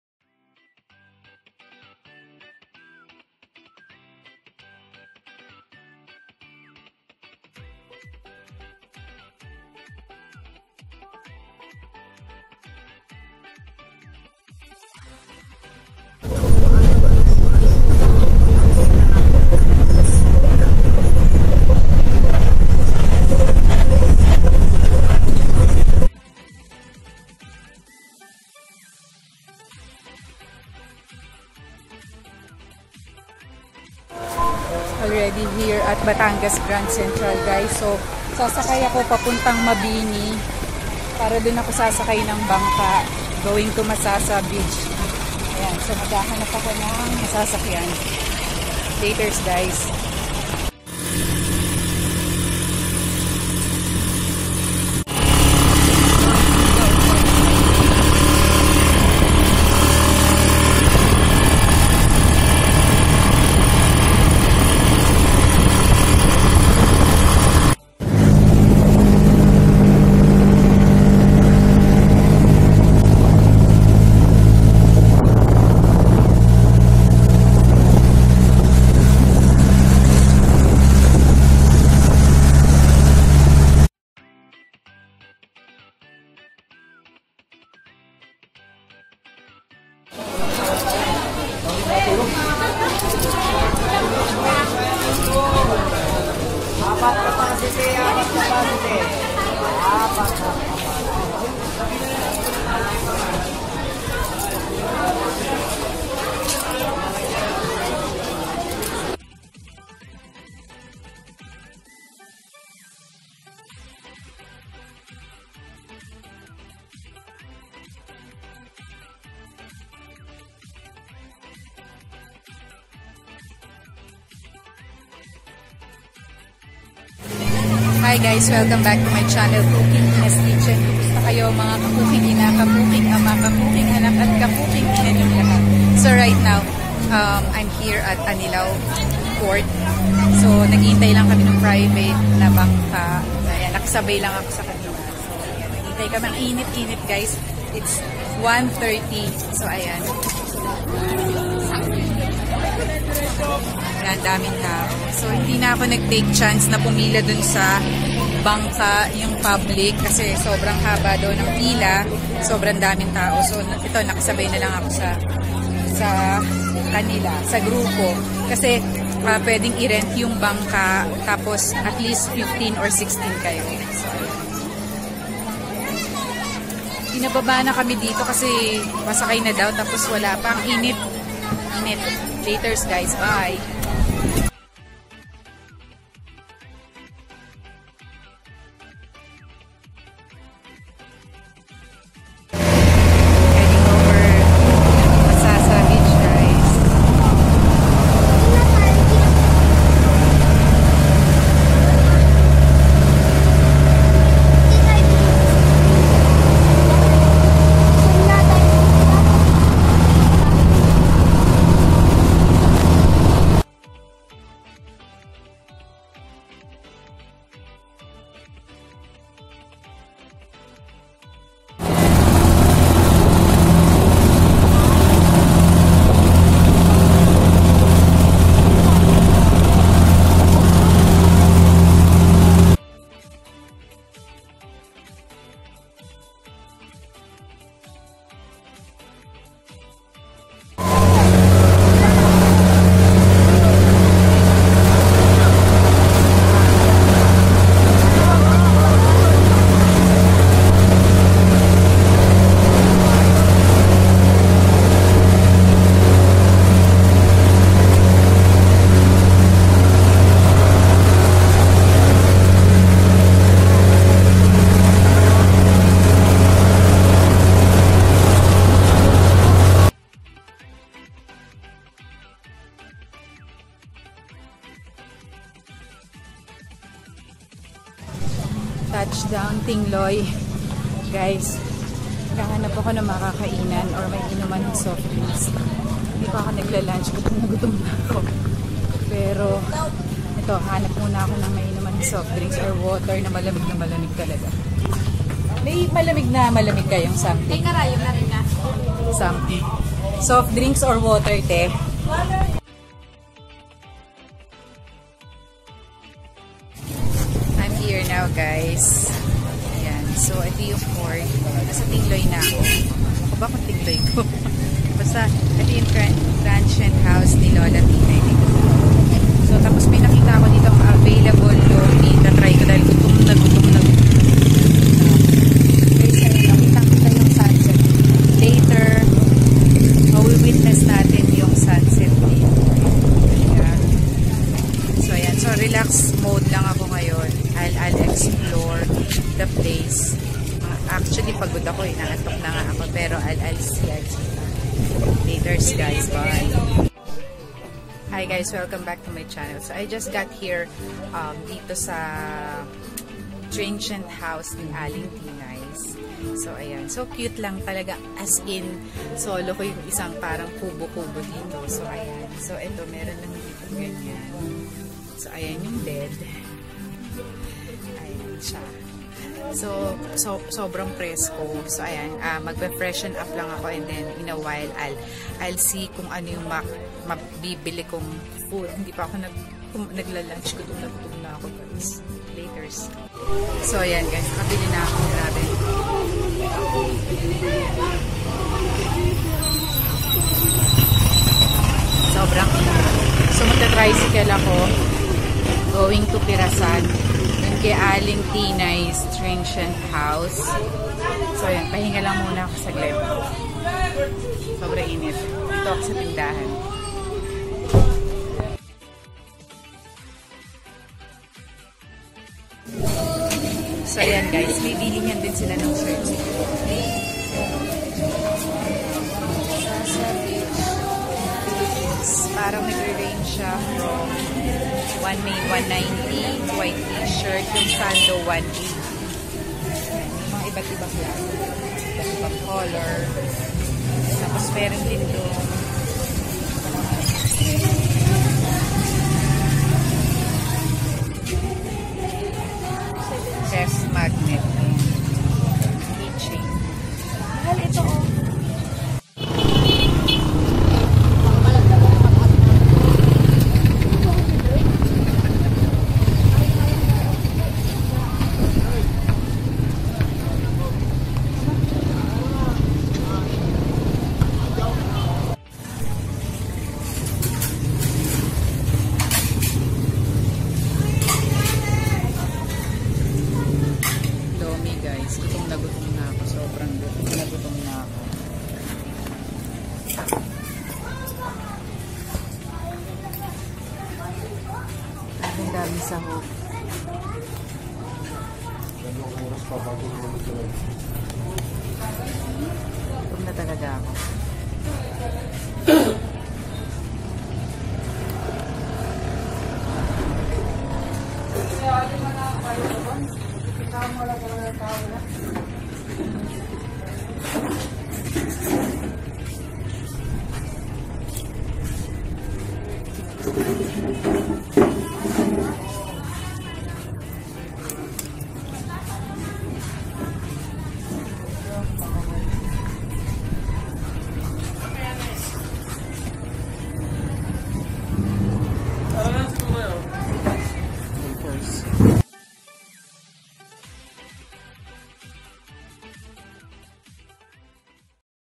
Vlog #182019 Day 1 at Masasa Beach, Tingloy Island Philippines
GRWM (Get Ready With Me) Masasa Beach, Tingloy Batangas Philippines
Food and Travel Vlog
April 18-21, 2019